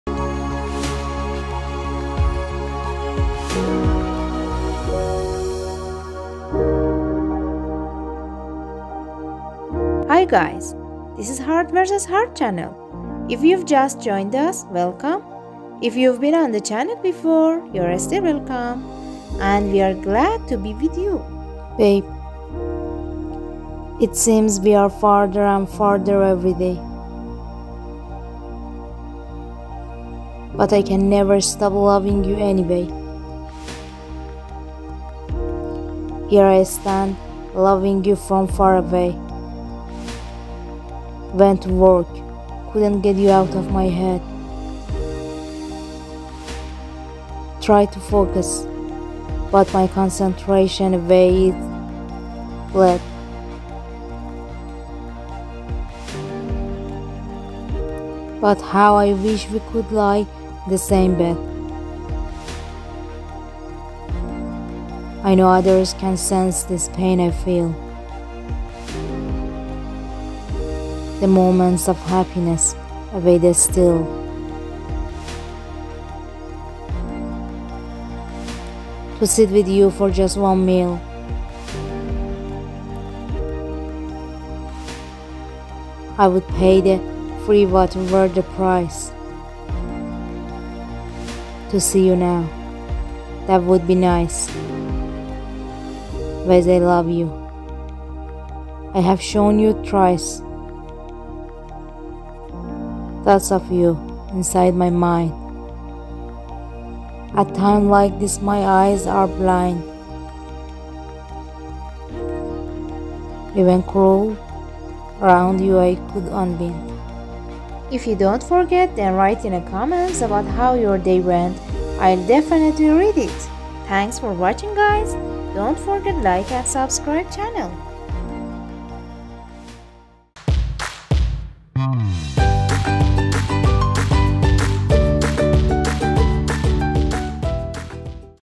🦋 dm to df message 🥵💌🥀 My love lasts forever, but you must listen to this message
Text👇 💞🧡💝
babe.
it seems we are farther and farther every day.
but I can never stop loving you anyway.
here I stand, loving you from … (Continue in video 😍💟)
playlist👇 Message
⚡ TimeStamps Chapters⚡
00:00 LOVE
00:01 beginning of love
00:54 Message
divine masculine  💟
dm df 🤍
dm message to df today 💝
dm tarot 💞